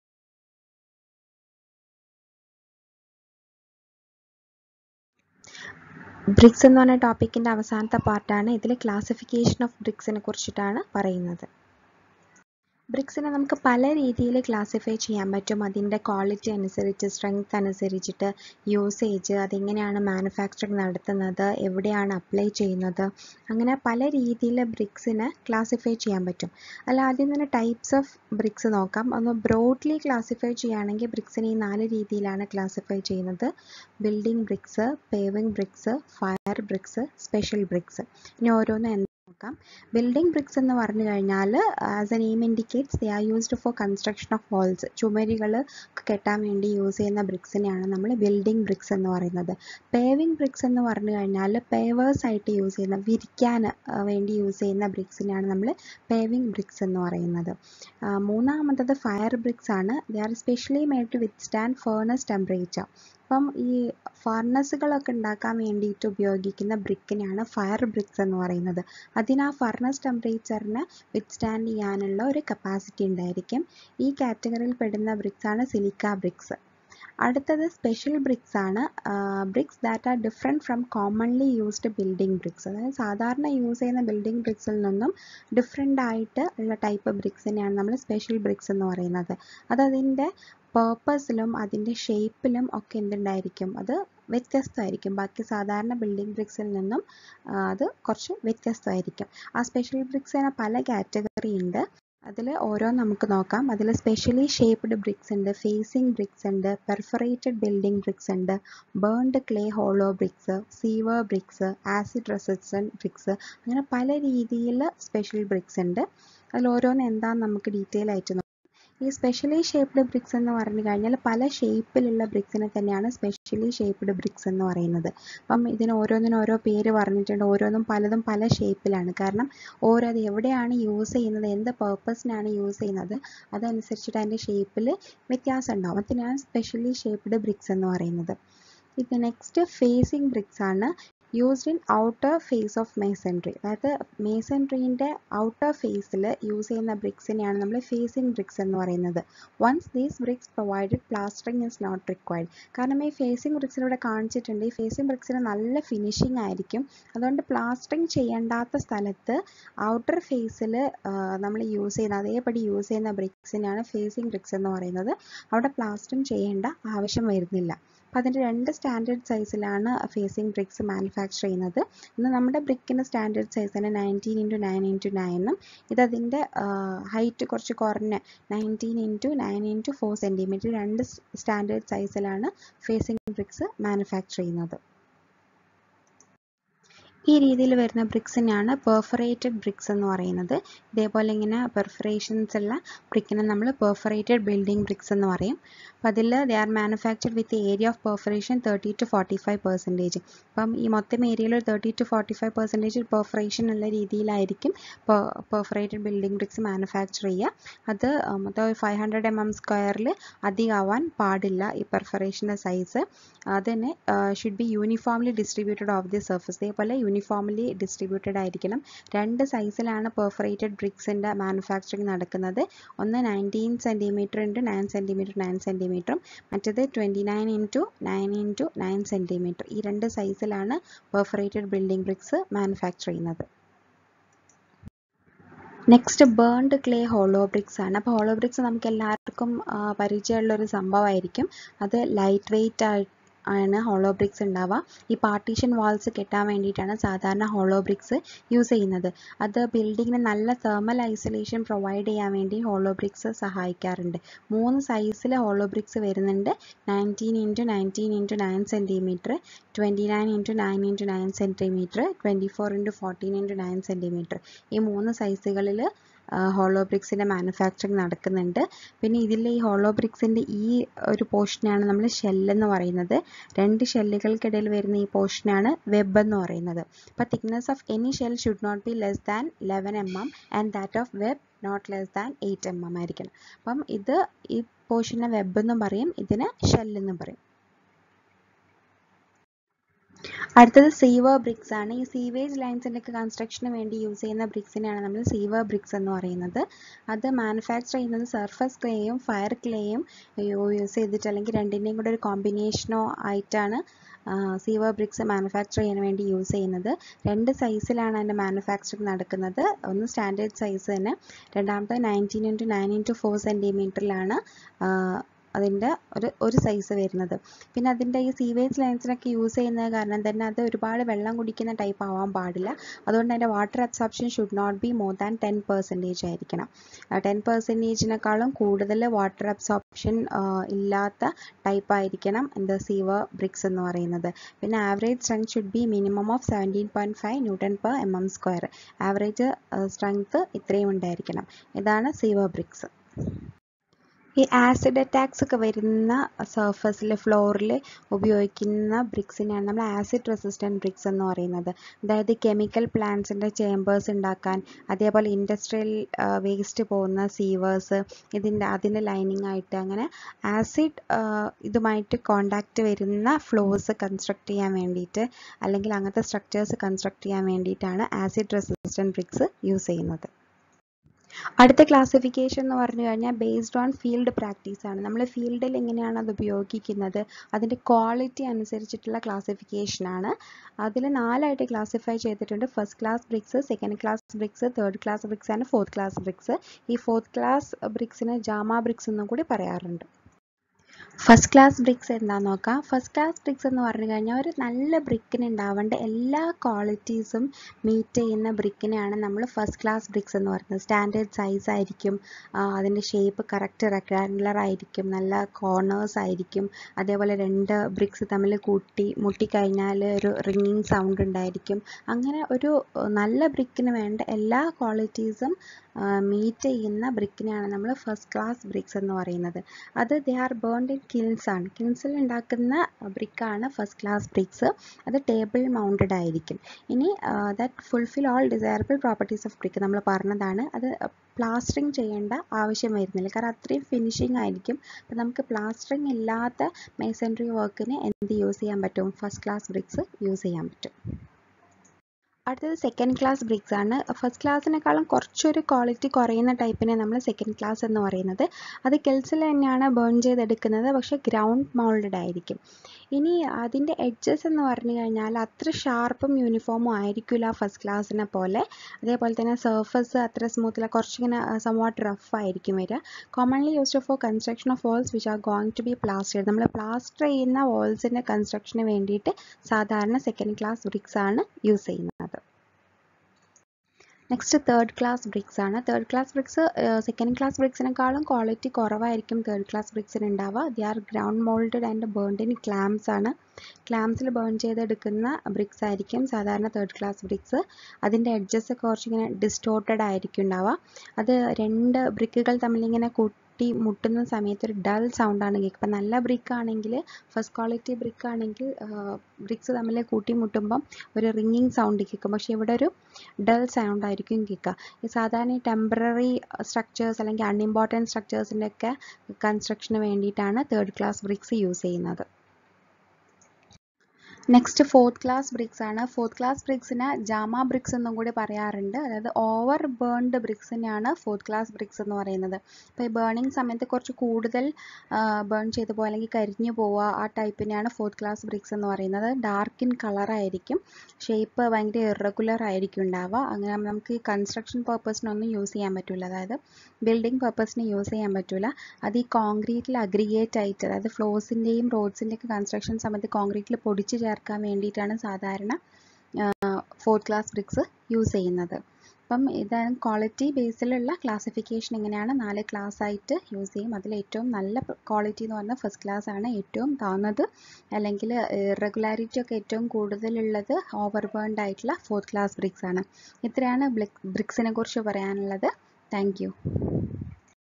Classification of Bricks|Basic civil Engg-part 3| Kerala PSC Lecturer in Computer, Mechanical Engg
Brief description about the classification of bricks.